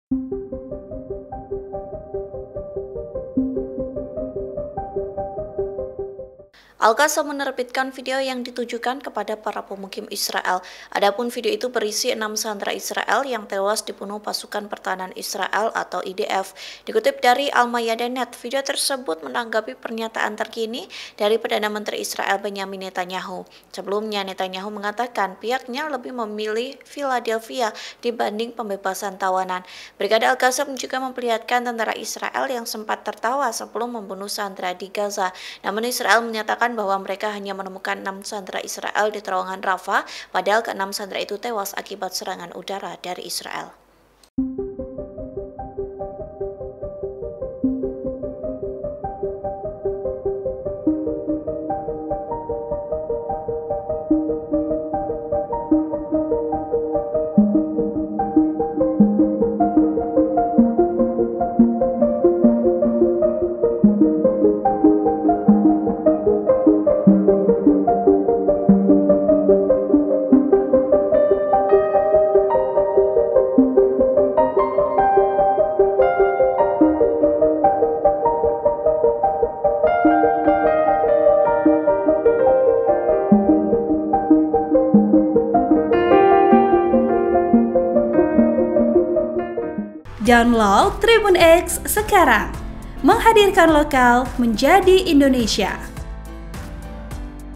Hamas Rilis Rekaman Video sebelum Tentara IDF Bunuh Sandera Israel di Gaza, Zionis Sempat Ketawa
TRIBUN-VIDEO.COM - Al Qassam menerbitkan video yang ditujukan kepada para pemukim Israel.

Adapun video itu berisi enam sandera Israel yang tewas dibunuh Pasukan Pertahanan Israel (IDF).

Dikutip dari Almayadeen Net, video tersebut menanggapi pernyataan terkini Perdana Menteri Israel Benjamin Netanyahu.

Sebelumnya, Netanyahu mengatakan pihaknya lebih memilih Philadelphia dibanding pembebasan tawanan.

Brigade Al Qasam juga memperlihatkan tentara Israel sempat tertawa sebelum membunuh sandera di Gaza.

Namun Israel menyatakan bahwa mereka hanya menemukan enam sandera Israel di terowongan Rafah.

Padahal keenam sandera itu tewas akibat serangan udara Israel.

(Tribun-Video.com)

Program: Tribunnews Update
Host: Maria Nanda Ayu Saputri
Editor Video: Ni'amu Shoim Assari Alfani
Uploader: bagus gema praditiya sukirman